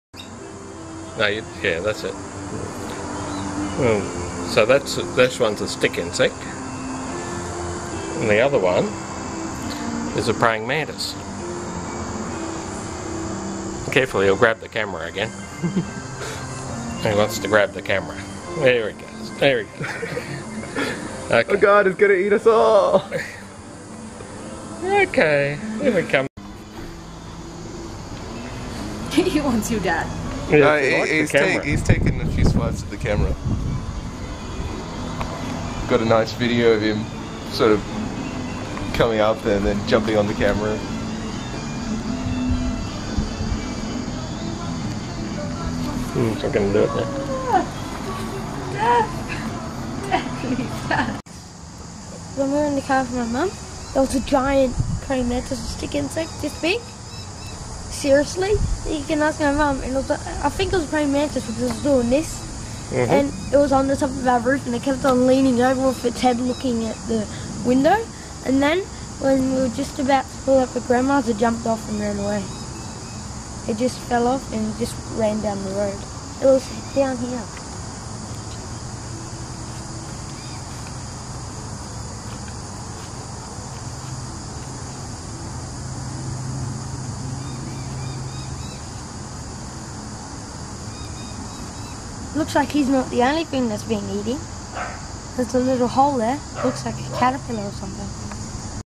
Playing with Goliath Stick Insect
Goliath Stick Insects are kept as exotic pets in some countries like the UK. Living beside Mt Gravatt Conservation Reserve these amazing creatures are just part of our bushland setting.